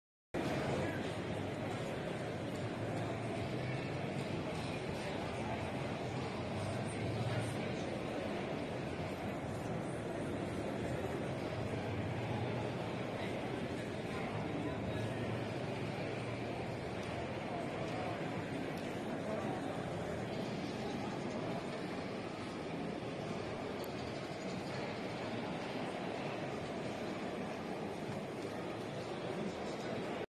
Friday 28th June 2024 Bovington Tank Museum, Dorset, England. 2024 "Tankfest" 3 Day Event.
Friday 28th June 2024 Bovington Tank Museum, Dorset, England. 2024 "Tankfest" 3 Day Event. Main complex and the world's only operational Tiger I, just returned from its loan period across Europe.